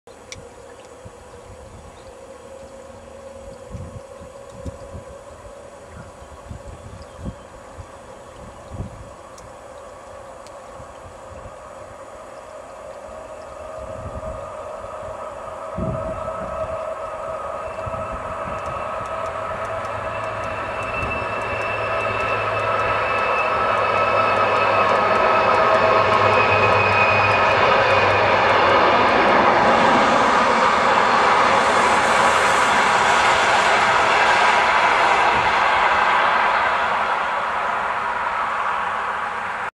Express train 707 passes railway bridge over route 87
Finnish Express train 707 passes railway bridge over route 87 on Pihlajanharju, Iisalmi, Finland.